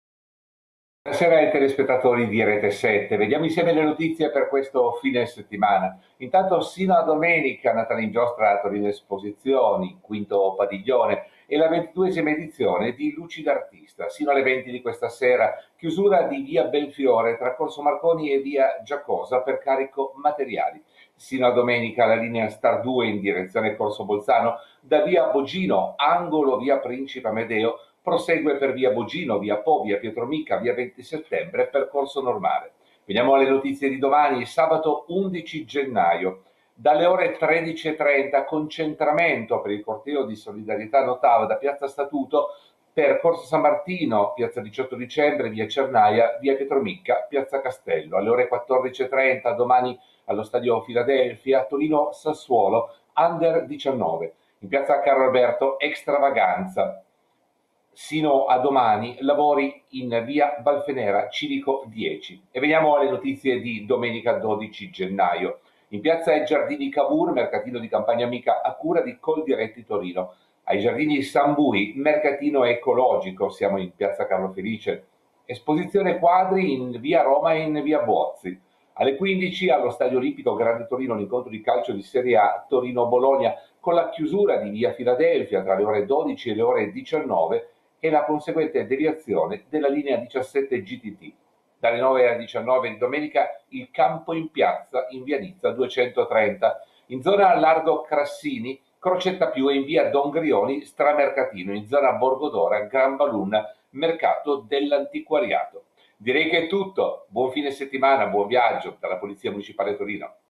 VALTER GERBI: TRAFFICO, CANTIERI ED EVENTI IN CITTA’
In collegamento con Valter Gerbi dalla Centrale Operativa dei vigili urbani di Torino, per tutti gli aggiornamenti su cantieri, viabilità ed eventi previsti in città per le prossime ore